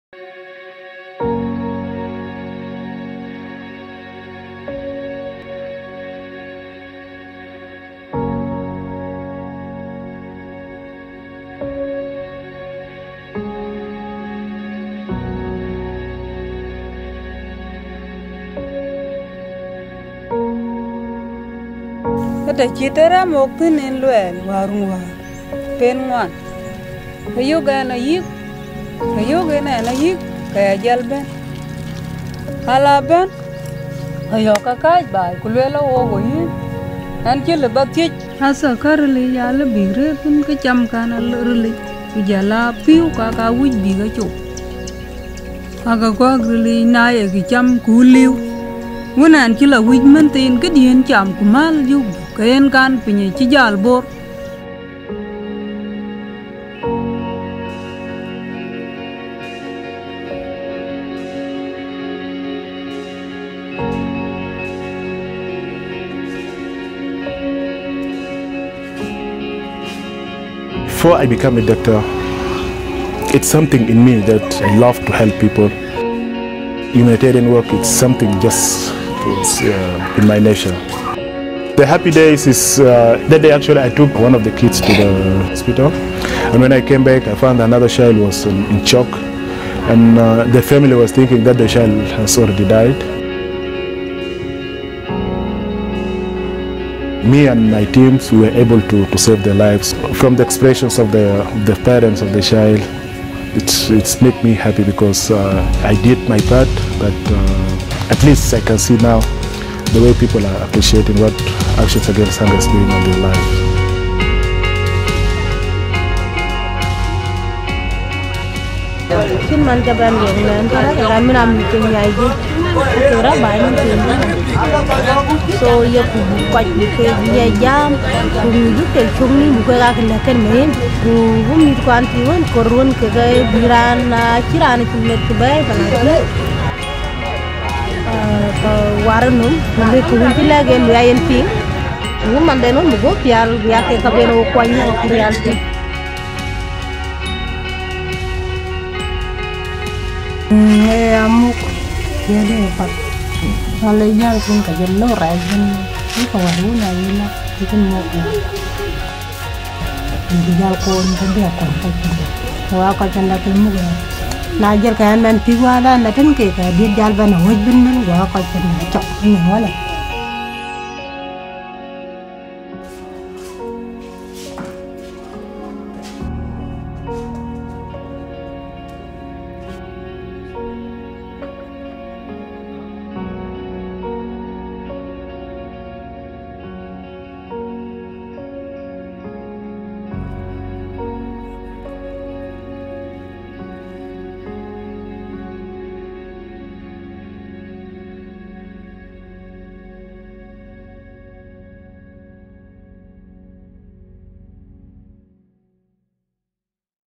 Sudán del Sur: Cuatro agentes de cambio
4 agentes de cambio en SudándelSur trabajan para combatir una crisis nutricional agravada por 4 años de conflicto: una voluntaria de Acción contra el Hambre, un doctor, una madre líder comunitaria y una emprendedora